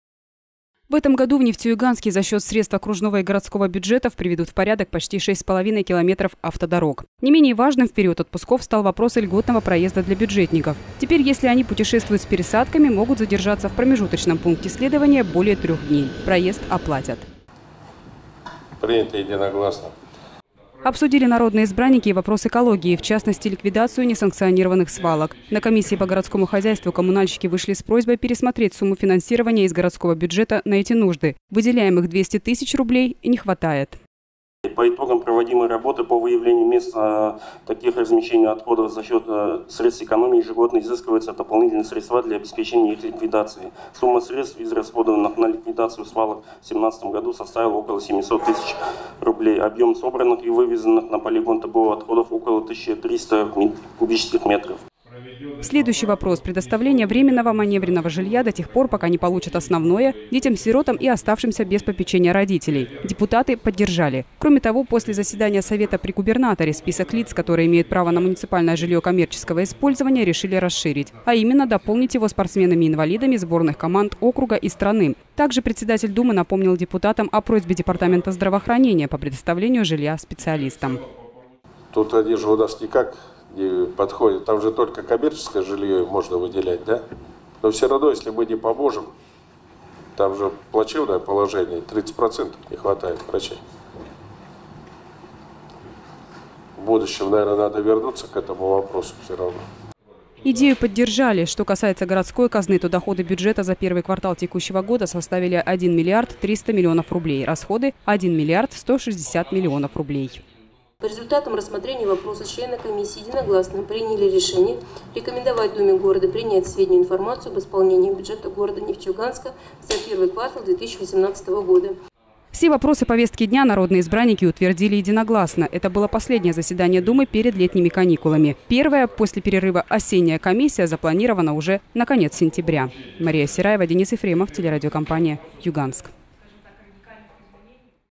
Последняя перед каникулами.
Последняя перед каникулами. 25 вопросов рассмотрели народные избранники на очередном заседании Думы. Подробно каждый доклад - депутаты обсуждали ранее на профильных комиссиях. А уже сегодня уже принимали конкретное решение - голосовали "За" или "Против".